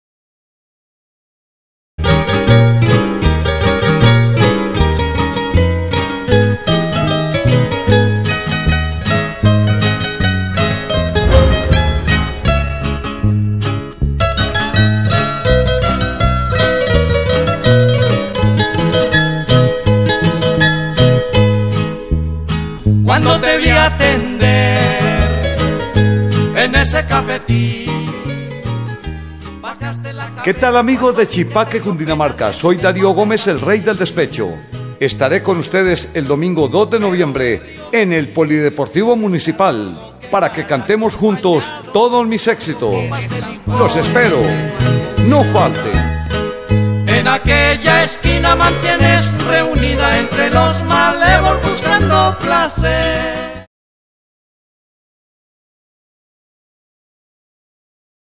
DARIO GOMEZ EN CHIPAQUE 2 DE NOVIEMBRE
PRIMER ARTISTA CONFIRMADO PARA LAS FERIAS Y FIESTAS, 2 DE NOVIEMBRE PLAZA DE TOROS, POR PRIMERA VEZ EL REY DEL DESPECHO EN CHIPAQUE !!!